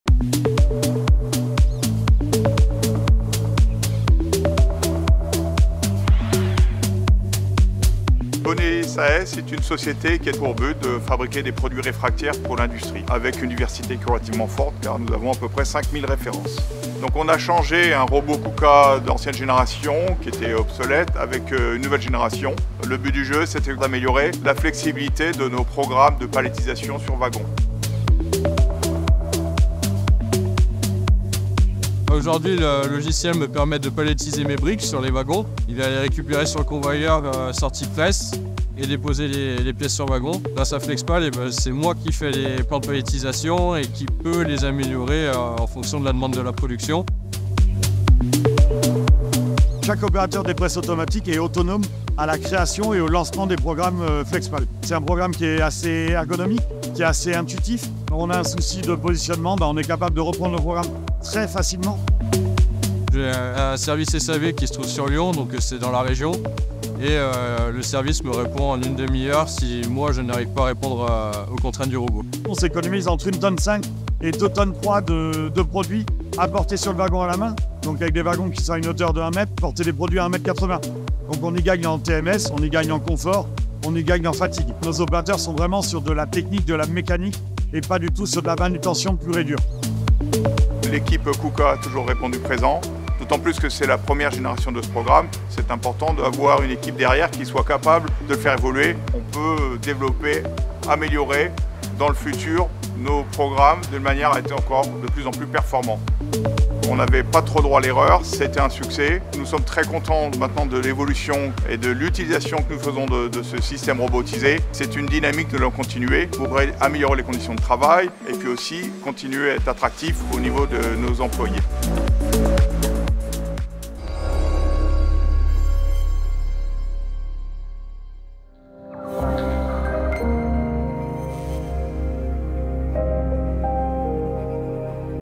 Easy palletizing with KUKA.FlexPal for refractory brick production
BONY SAS, a manufacturer of refractory bricks for industrial furnaces, has optimized its production through the use of FlexPal easy palletizing software.
The simple 3D modeling of pallets, bricks and grippers enables line operators to be fully autonomous in modifying palletization plans.
No programming knowledge is required, as the software manages trajectories and multi-layering. Trajectories are then imported into the robot, and no points are defined on the robot.